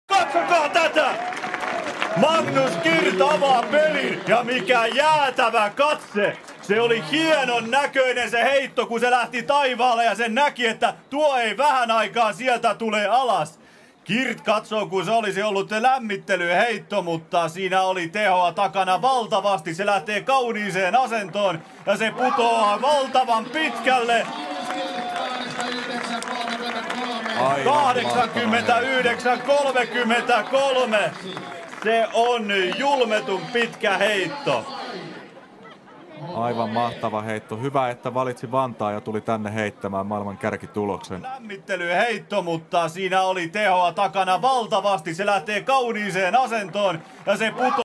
Magnus Kirt | 89,33M | 2019 world Javelin lead!
Magnus Kirt - 89,33M - World Lead - Men's Javelin - Kultainen Keihäs 2019